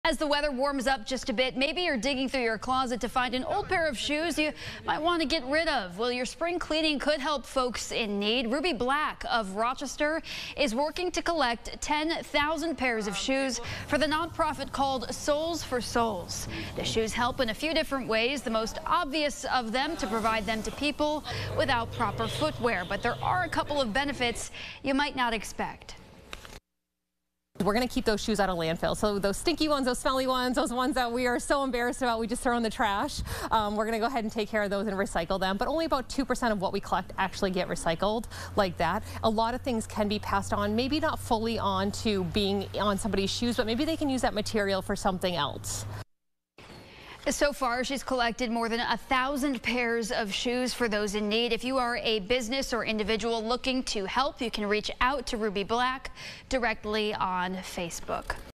Rochester woman collecting 'Soles4Souls'
As the weather warms up, maybe you're digging through your closet to find an old pair of shoes you want to get rid of. That spring cleaning could help folks in need.